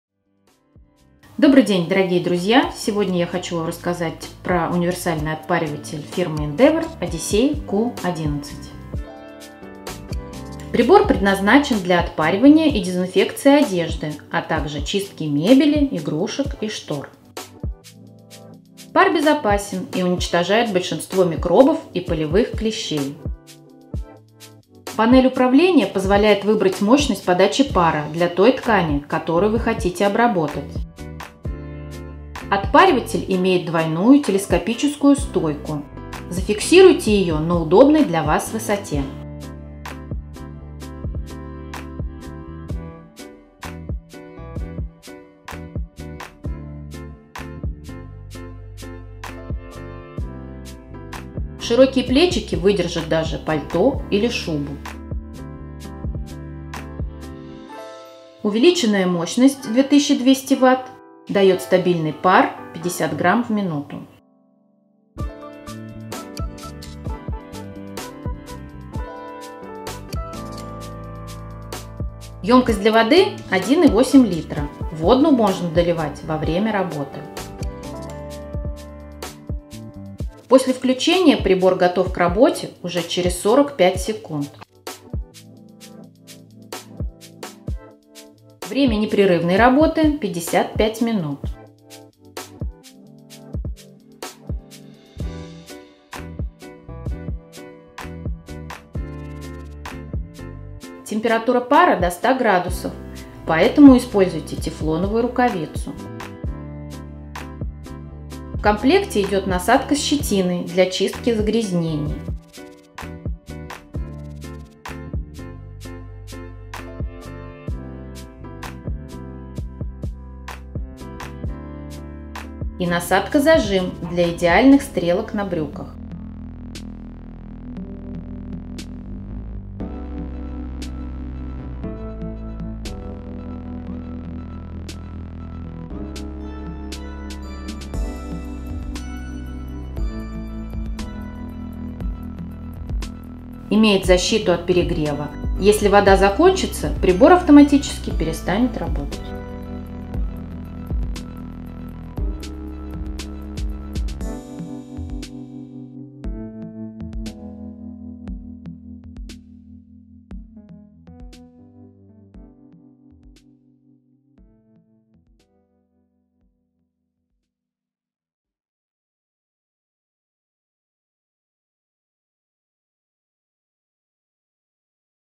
Универсальный отпариватель ENDEVER ODYSSEY Q-11
Бренд: ENDEVER
Максимальная мощность (Вт): 2200
Номинальное напряжение (В): 220 В, 50 Гц
Производительность пара (г/мин): 50
Максимальная температура пара (°С) : 95 ±5°
Объем резервуара для воды (л): 1,8
Готовность к работе (сек): 45
Время непрерывной работы (мин): 55
Высота стойки (см): 175
Длина шнура (м): 1,8
Габаритные размеры: 30*27.5*21.5
Цвет: белый / бордовый
Вес изделия (кг): 4,1

Особенности и дополнительные функции:
Сенсорная панель управления
Защита от скачков напряжения сети
Абсолютно безопасен: имеет защиту от перегрева, которая, в случае повреждения термостата или отсутствия воды в баке, автоматически отключает прибор от электрической сети
Регулировка мощности пара
Съемный бак
Устойчивая двойная телескопическая стойка
Индикатор готовности к работе

Комплект поставки:
Отпариватель
Универсальная щетка-насадка с длинной полимерной щетиной
Вешалка, держатель распылителя
Насадка зажим для наведения стрелок на брюках
Двойная телескопическая стойка
Гарантийный талон
Руководство по эксплуатации

Гарантия (мес.): 18